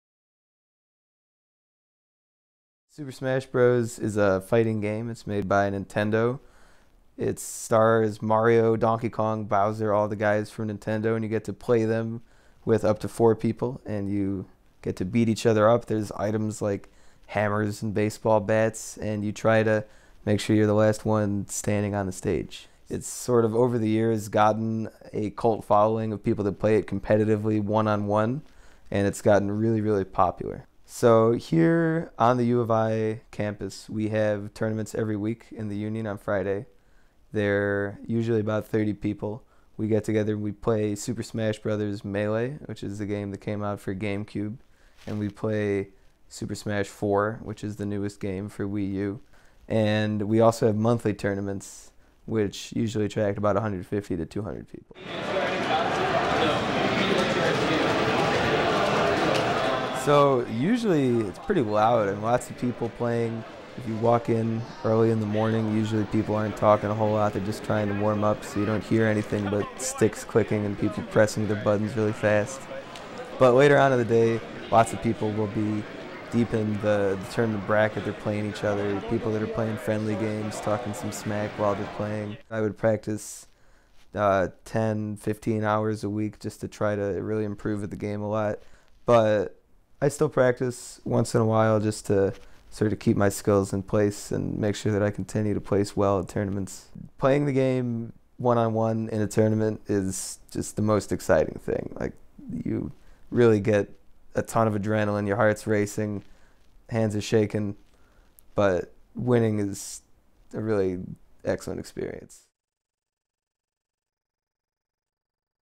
Super Smash Bros Tournament
Max McKittrick talks about the Smashing Illini Registered Student Organization at UIUC. They have weekly melees and monthly tournaments.